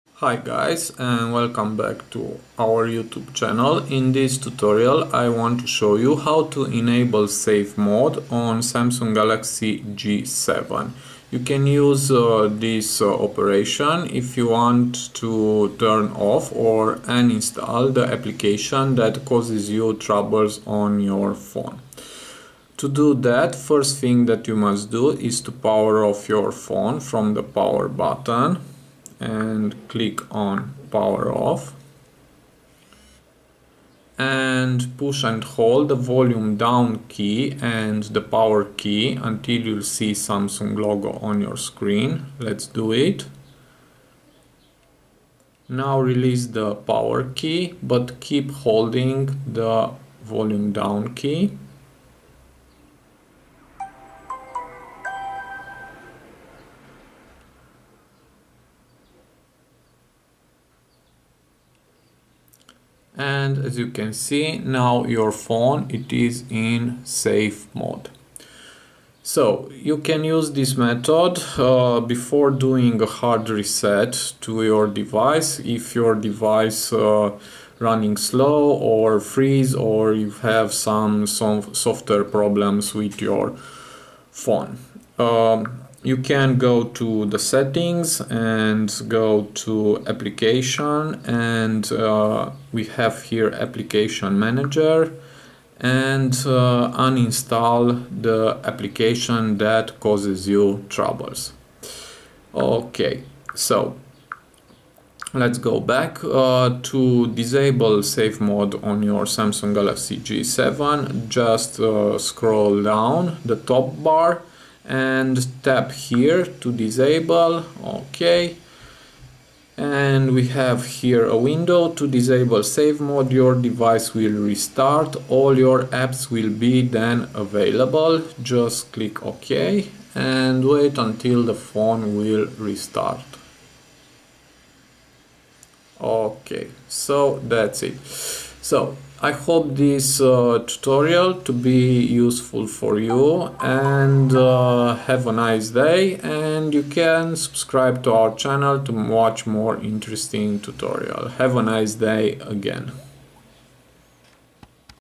How to enable SAFE MODE on Samsung Galaxy J7
In this tutorial I will show you how to enable and disable SAFE MODE on Samsung Galaxy J7 J700F. You can use this option if you want to turn off and uninstall the apps that causing troubles on your phone.
IMPORTANT
Safe mode allows to your device to run without non-essential applications.
Use this method to see what app is causing your device to freeze/reset/run slow.
Use this method before any hard reset.
This operation also should work on:J700M (LATAM); J700H with no LTE (South Africa, Pakistan, Vietnam)